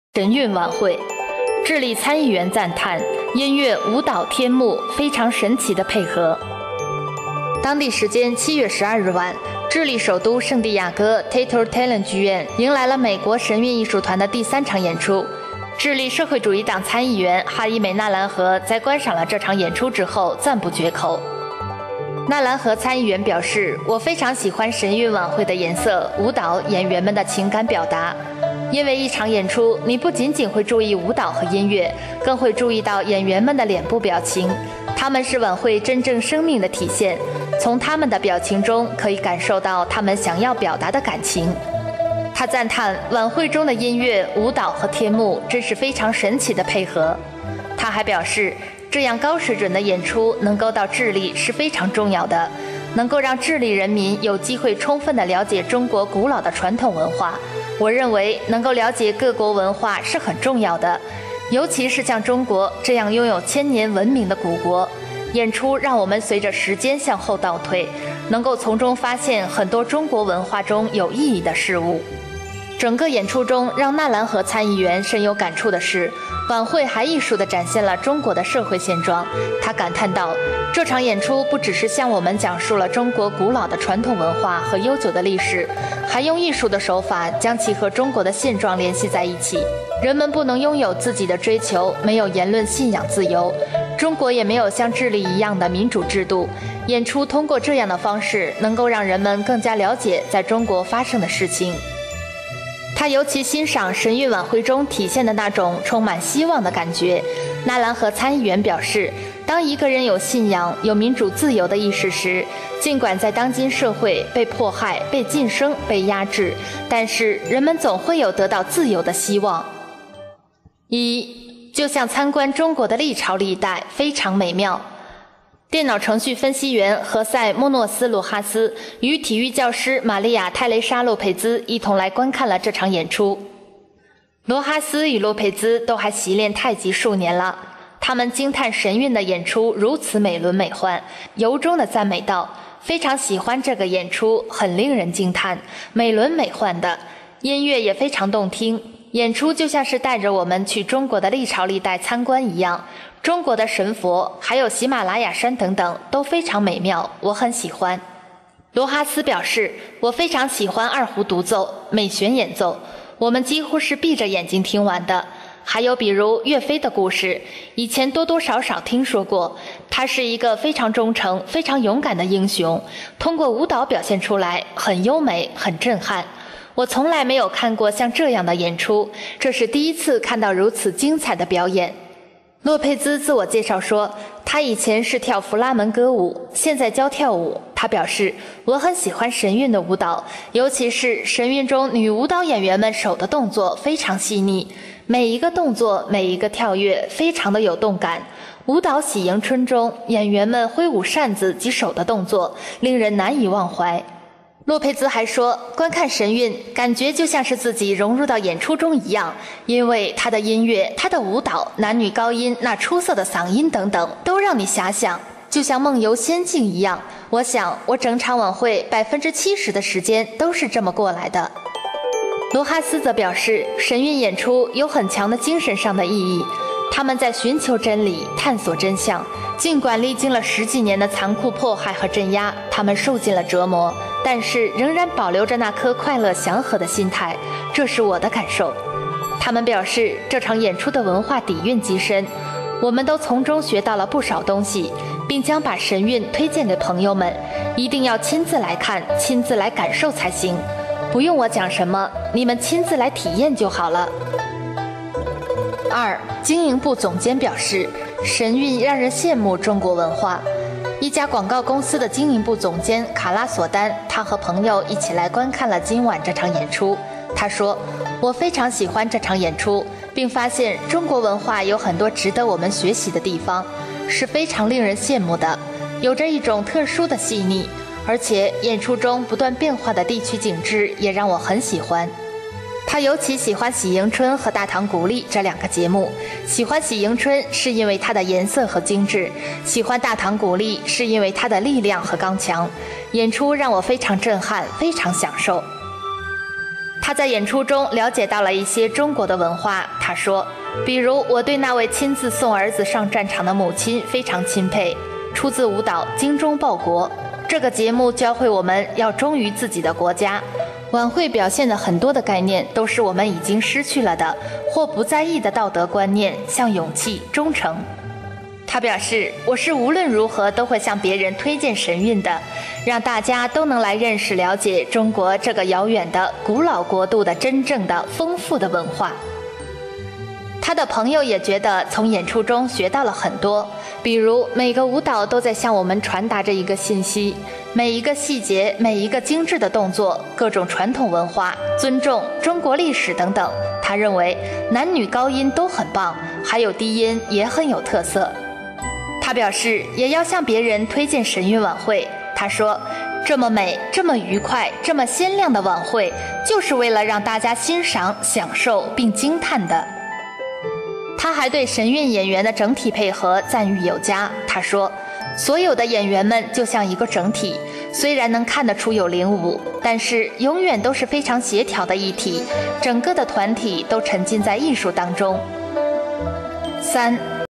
智利參議員：音樂舞蹈天幕非常神奇的配合 上
智利參議員：音樂舞蹈天幕非常神奇的配合

【正見網2009年07月14日】
參議員：音樂舞蹈天幕非常神奇的配合
當地時間7月12日晚，智利首都聖地亞哥Teatro Teleton劇院迎來了美國神韻藝術團的第三場演出。智利社會主義黨參議員哈伊美.納蘭何在觀賞了這場演出之後，讚不絕口。

納蘭何參議員表示，「我非常喜歡（神韻）晚會的顏色，舞蹈，演員們的情感表達，因為一場演出你不僅僅會注意舞蹈和音樂，更會注意到演員們的臉部表情，他們是晚會真正生命的體現。從他們的表情中可以感受到他們想要表達的感情。」

他讚歎「（晚會中）的音樂，舞蹈和天幕真是非常神奇的配合。」

他還表示，「這樣高水準的演出能夠到智利是非常重要的，能夠讓智利人民有機會充分的瞭解中國古老的傳統文化。我認為能夠瞭解各國文化是很重要的，尤其是像中國這樣的擁有千年文明的古國。演出讓我們隨著時間向後倒退，能夠從中發現很多中國文化中有意義的事物。」

整 個演出中讓納蘭何參議員深有感觸的是，晚會還藝術的展現了中國的社會現狀。他感歎道：「這場演出不只是向我們講述了中國古老的傳統文化和悠久的歷史，還用 藝術的手法將其和中國的現狀聯繫在一起，人們不能擁有自己的追求，沒有言論信仰自由，中國也沒有像智利一樣的民主制度。演出通過這樣的方式能夠讓人們更加 瞭解在中國發生的事情。

他尤其欣賞神韻晚會中體現的那種「充滿希望的感覺」，納蘭何參議員表示，「當一個人有信仰，有民主自由的意識時，儘管在當今社會被迫害，被噤聲，被壓制，但是人們總會有得到自由的希望。」
「就像參觀中國的歷朝歷代 非常美妙」

電腦程序分析員何塞•木諾斯•羅哈斯與體育教師瑪麗亞•泰蕾莎•洛佩茲一同來觀看了這場演出。
羅哈斯與洛佩茲還都練習太極數年了。他們驚歎神韻的演出如此美輪美奐，由衷地讚美道：「非常喜歡這個演出，很令人驚歎，美輪美奐的，音樂也非常動聽，演出就像是帶著我們去中國的歷朝歷代參觀一樣，中國的神佛，還有喜馬拉雅山等等，都非常美妙，我們很喜歡。」
羅哈斯表示：「我非常喜歡二胡獨奏（美旋演奏），我們幾乎是閉著眼睛聽完的。還有比如岳飛的故事，以前多多少少聽說過。他是一個非常忠誠非常勇敢的英雄，通過舞蹈表現出來，很優美，很震撼。我從來沒有看過像這樣的演出，這是第一次看到如此精彩的表演。」
洛佩茲自我介紹說，她以前是跳弗拉門戈舞，現在教跳舞。她表示「我很喜歡（神韻）舞蹈，尤其是神韻中女演員們手的動作非常細膩，每一個動作，每一個跳躍，非常的有動感。」舞蹈《喜迎春》中，演員們揮舞扇子及手的動作，令人難以忘懷。
洛佩茲還說，觀看神韻，「感覺就像是自己融入到演出中一樣，因為他的音樂，他的舞蹈，男女高音，那出色的嗓音等等都讓你遐想，就像夢遊仙境一樣。我想我整場晚會70%的時間都是這麼過來的。」
羅哈斯則表示，神韻演出「有很強的精神上的意義，他們在尋求真理，探索真相，儘管經歷了十幾年的殘酷迫害和鎮壓，他們受盡了折磨，但是仍然保留著那顆快樂祥和的心態，這是我的感受。」
他們表示這場演出的文化底蘊極深。「我們都從中學到了不少東西」，並將把神韻推薦給朋友們，「一定要親自來看，親自來感受才行，不用我講什麼，你們親身來體驗就好了。」
(未完)原文請看